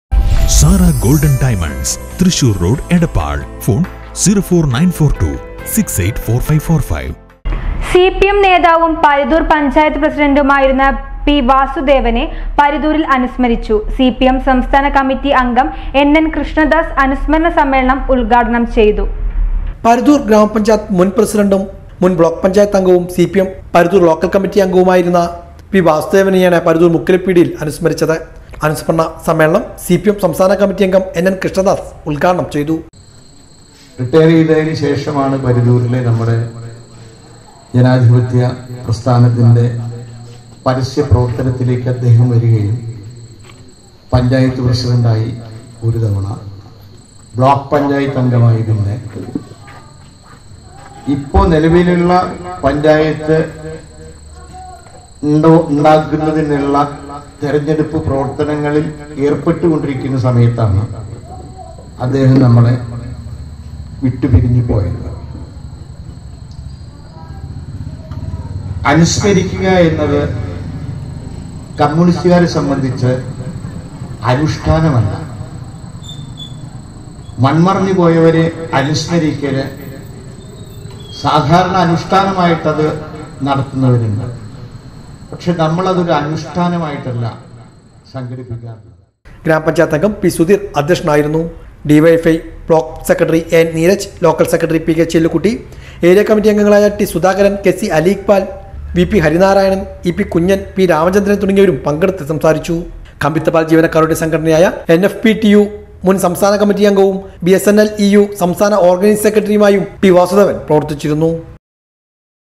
സി.പി.എം.നേതാവും പരുതൂർ പഞ്ചായത്ത് പ്രസിഡന്റുമായിരുന്ന പി.വാസുദേവനെ പരുതൂരിൽ അനുസ്മരിച്ചു
സി.പി.എം.നേതാവും പരുതൂർ പഞ്ചായത്ത് പ്രസിഡന്റുമായിരുന്ന പി.വാസുദേവനെ പരുതൂരിൽ അനുസ്മരിച്ചു. സി.പി.എം.സംസ്ഥാന കമ്മിറ്റി അംഗം എൻ.എൻ.കൃഷ്ണദാസ് അനുസ്മരണ സമ്മേളനം ഉദ്ഘാടനം ചെയ്തു